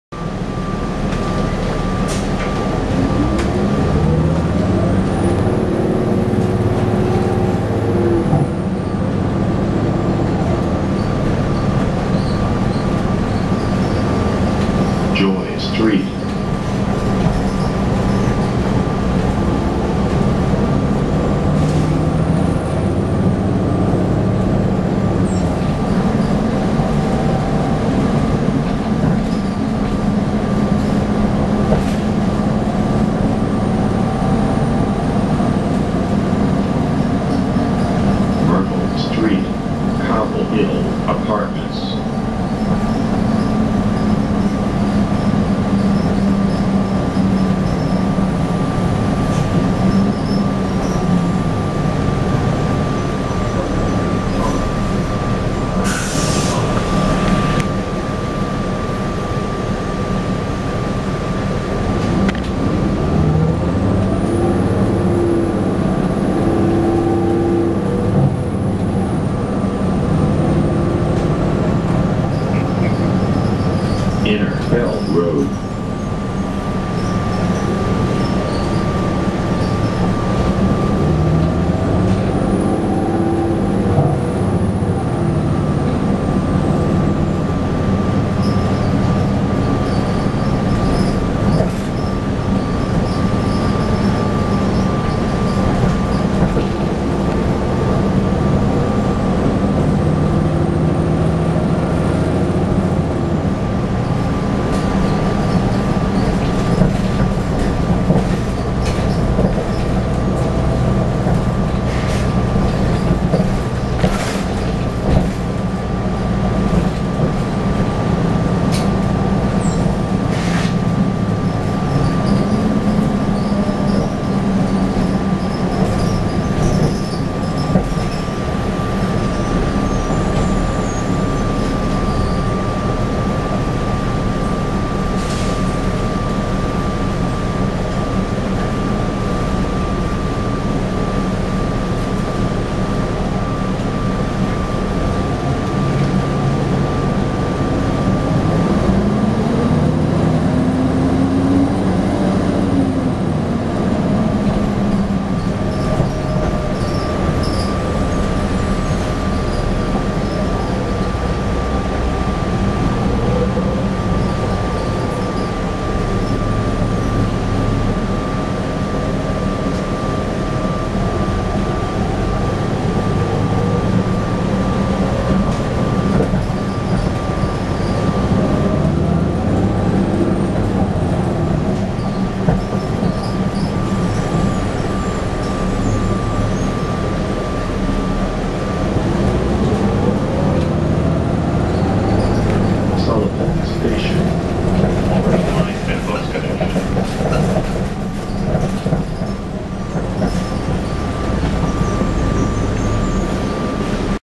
MBTA #0418 (The Ride)
On the CT3 line to Ruggles station.

This is a 2005 Neoplan AN440LF with a Catepillar C-9 engine and Allison B400R transmission.